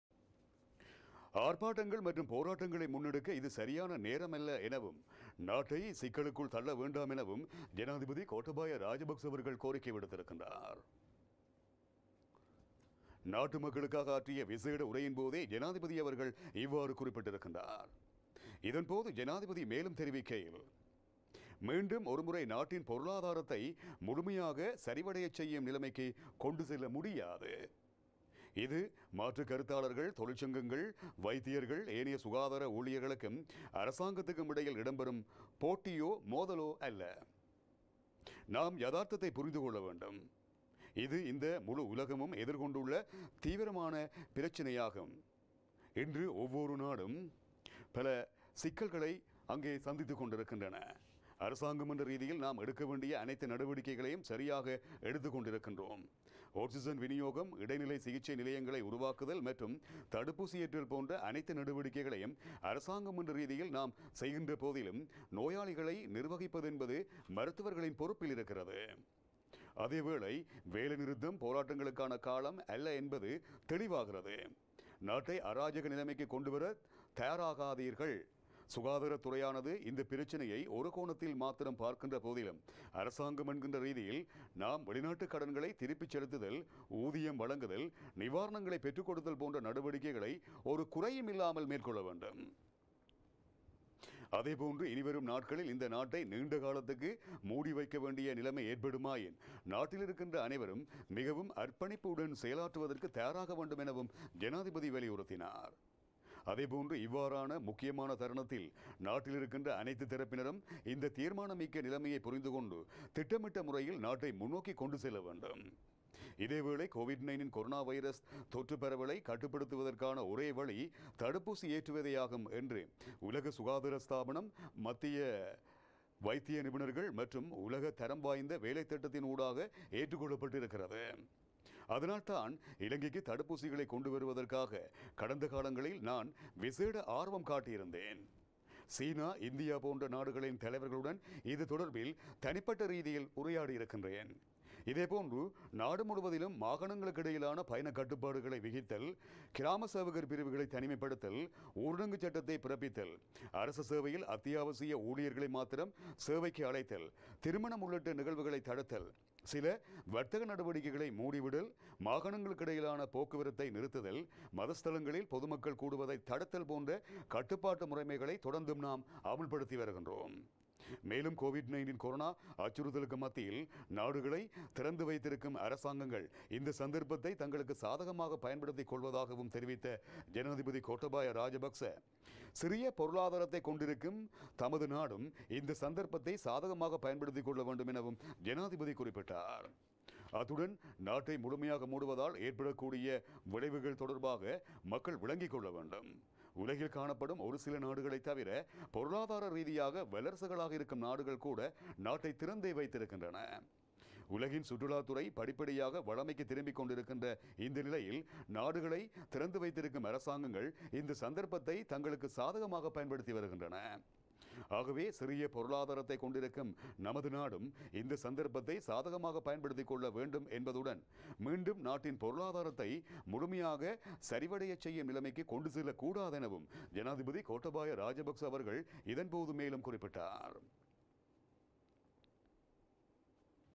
ஆர்ப்பாட்டங்கள் , போராட்டங்களை முன்னெடுக்க இது சரியான நேரம் அல்ல!
ஆர்ப்பாட்டங்கள் , போராட்டங்களை முன்னெடுக்க இது சரியான நேரம் அல்ல  - யதார்த்தத்தைப் புரிந்துகொள்ள வேண்டும் என ஜனாதிபதி கோட்டாபய ராஜபக்ச வலியுறுத்து!